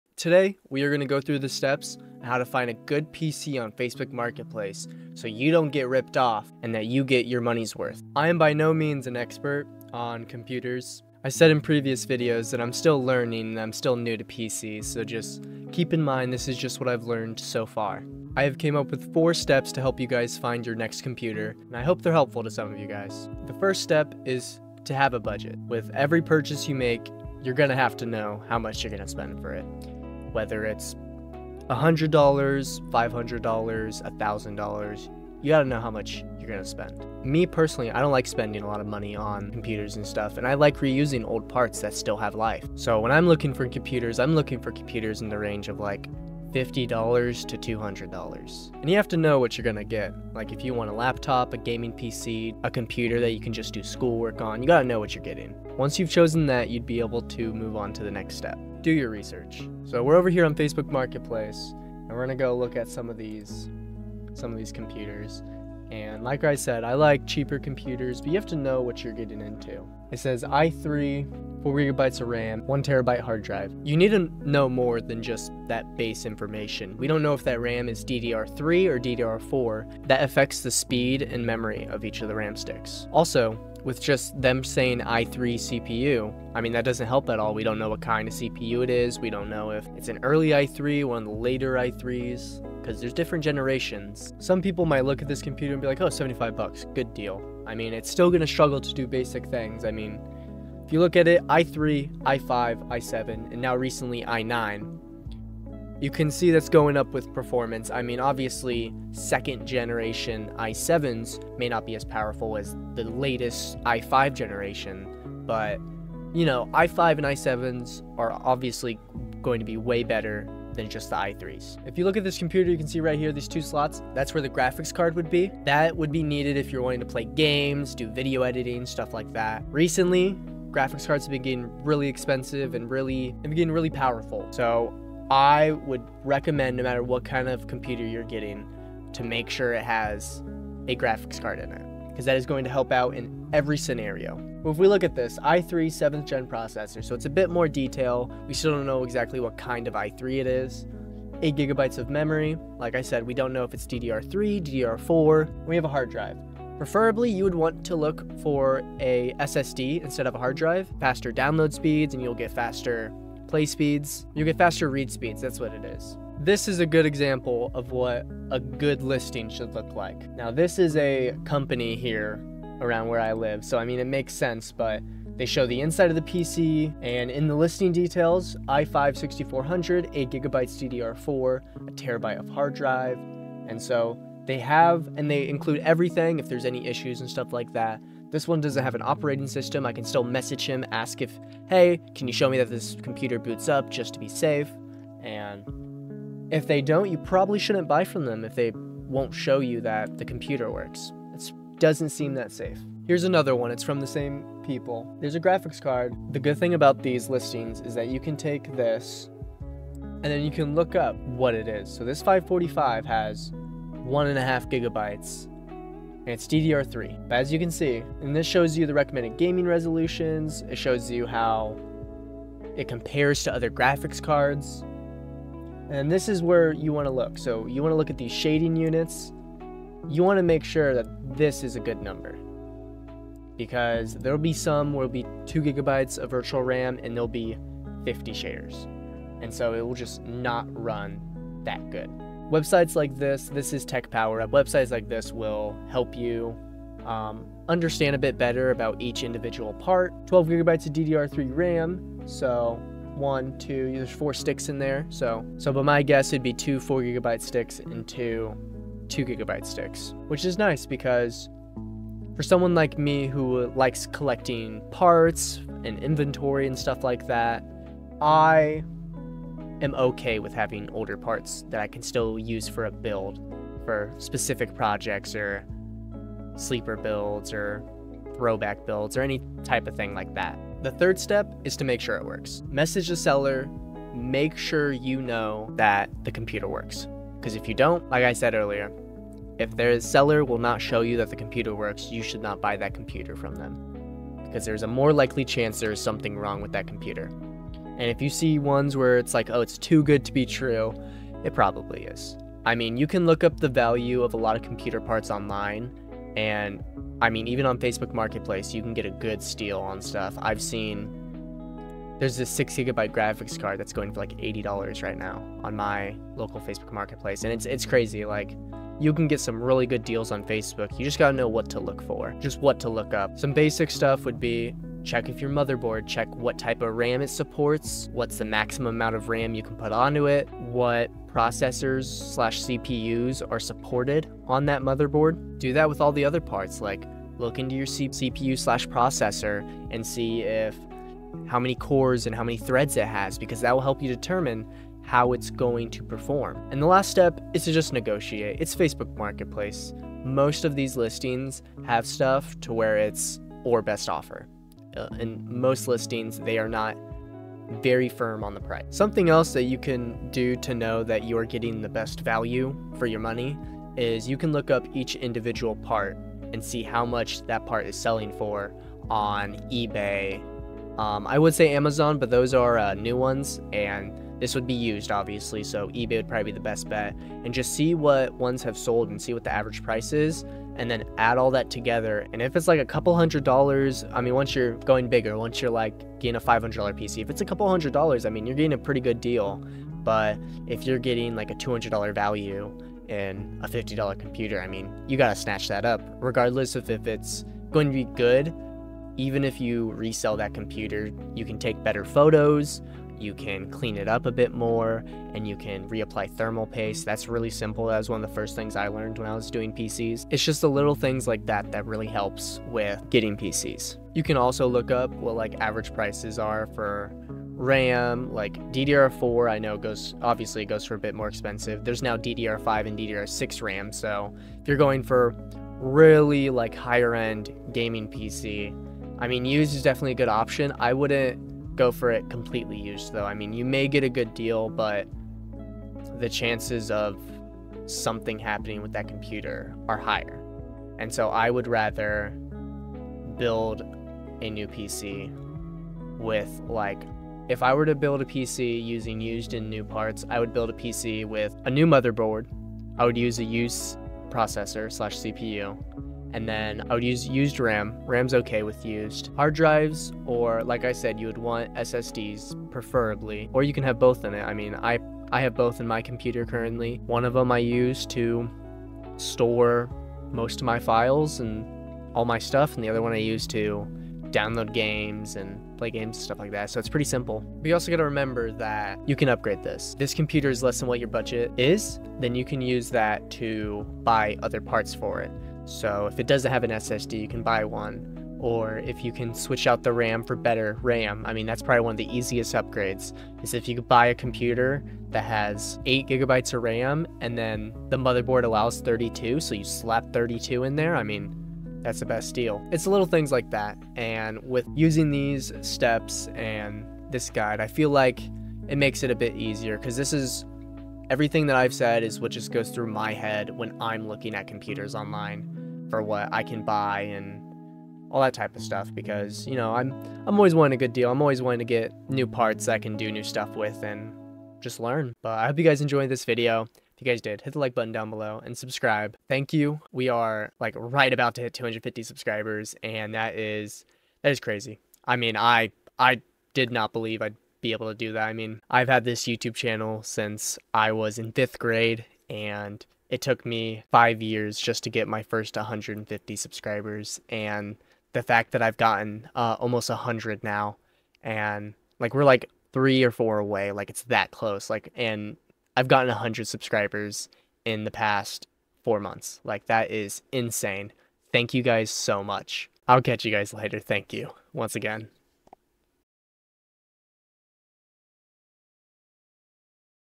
How To Find a Good PC on Facebook Marketplace!!
Today I will be showing you guys how I look for computers on Facebook Marketplace. I go into detail about things you should look for and avoid.